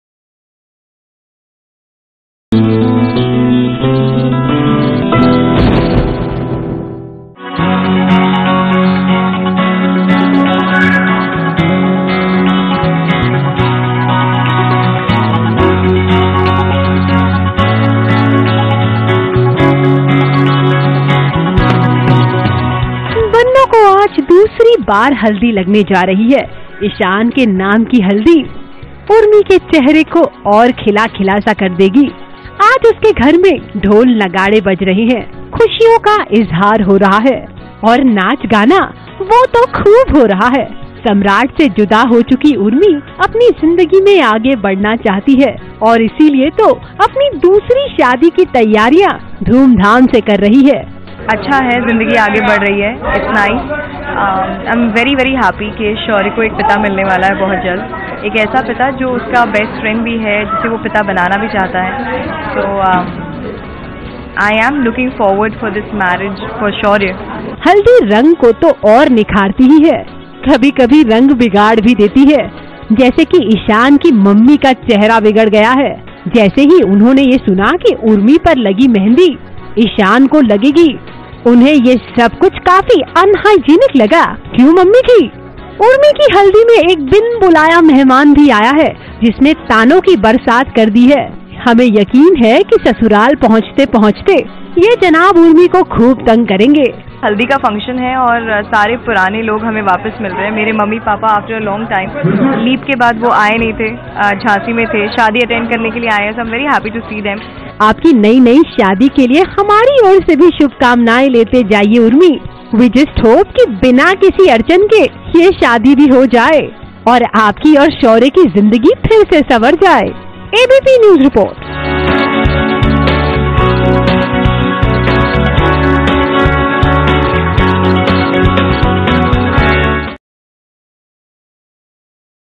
Urmi over her old life and looking forward for new marriage
Urmi is over her old life and is looking forward for new marriage. Who is the new guest in Urmi's marriage?